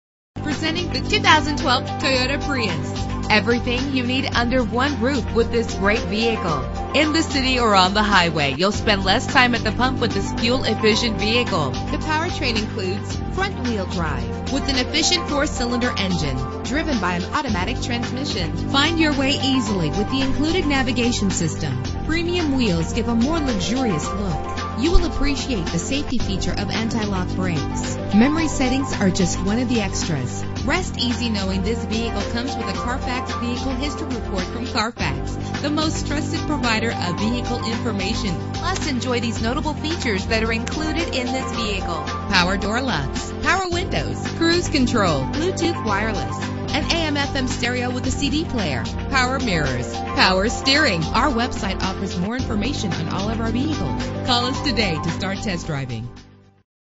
2012 Toyota Prius - Folsom CA
Year: 2012
Make: Toyota
Model: Prius
Trim: Three
Engine: 1.8 liter inline 4 cylinder 16 valve MPFI DOHC
Transmission: Automatic CVT
Color: White
Mileage: 43224
Address:
12755 Folsom Blvd.